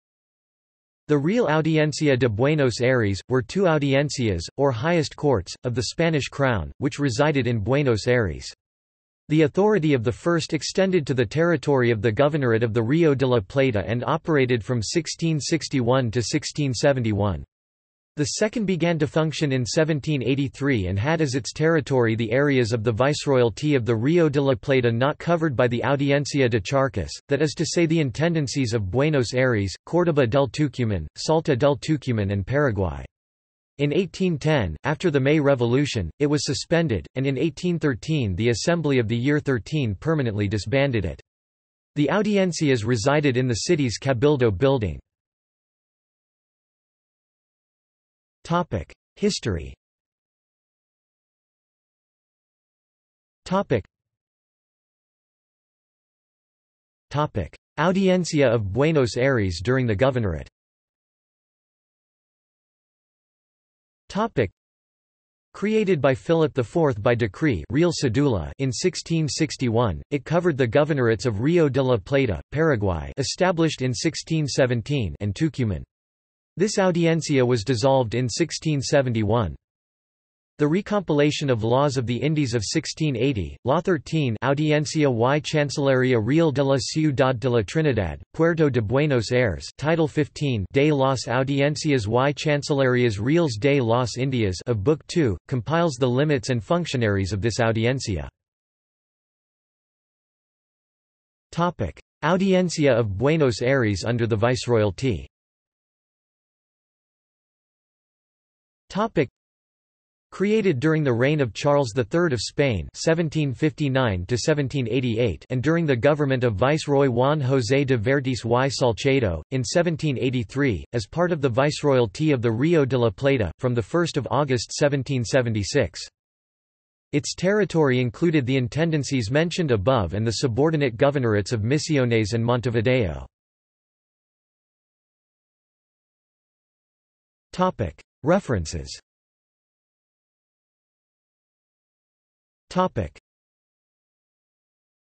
Royal Audiencia of Buenos Aires | Wikipedia audio article
This is an audio version of the Wikipedia Article:
Royal Audiencia of Buenos Aires

SUMMARY
The Real Audiencia de Buenos Aires, were two audiencias, or highest courts, of the Spanish crown, which resided in Buenos Aires. The authority of the first extended to the territory of the Governorate of the Río de la Plata and operated from 1661 to 1671. The second began to function in 1783 and had as its territory the areas of the Viceroyalty of the Río de la Plata not covered by the Audiencia de Charcas, that is to say the intendancies of Buenos Aires, Córdoba del Tucumán, Salta del Tucumán and Paraguay. In 1810, after the May Revolution, it was suspended, and in 1813 the Assembly of the Year XIII permanently disbanded it. The Audiencias resided in the city's cabildo building.